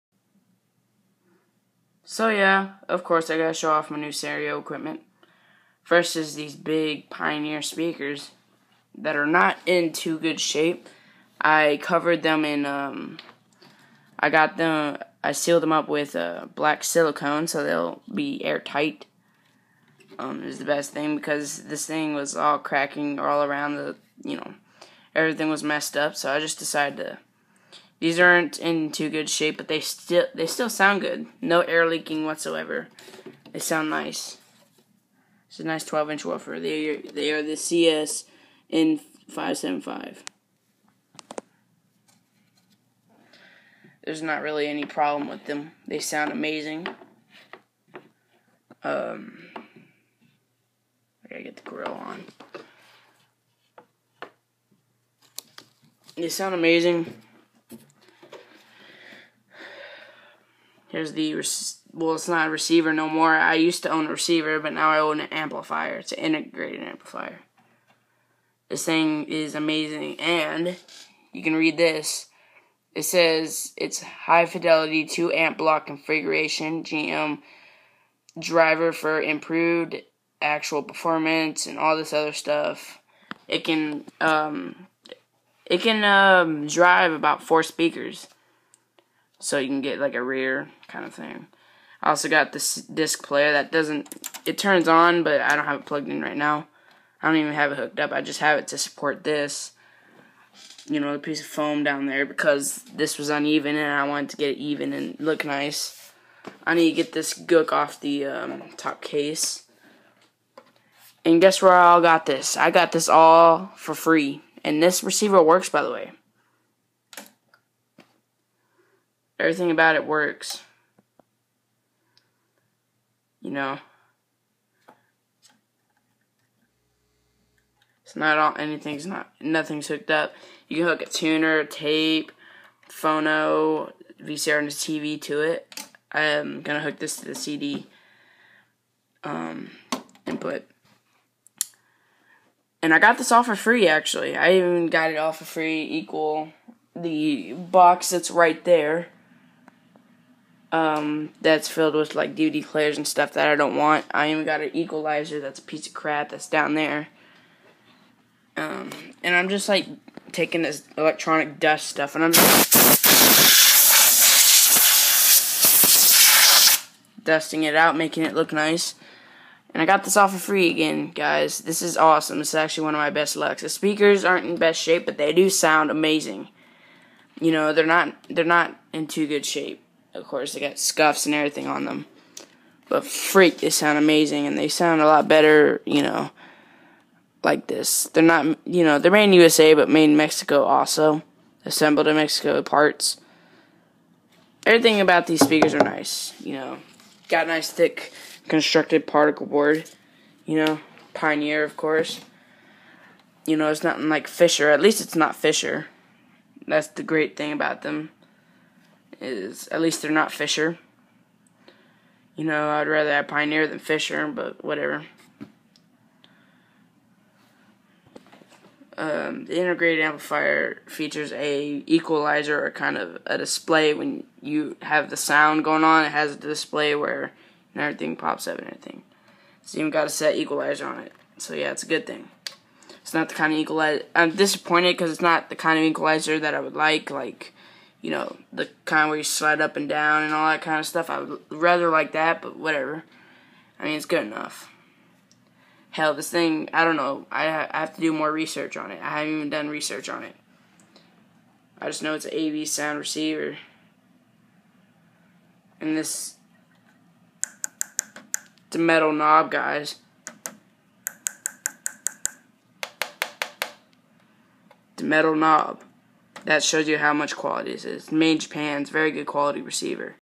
A stereo I got for free
I happen to come across some equipment that I got for free from one of my friends it sounds amazing!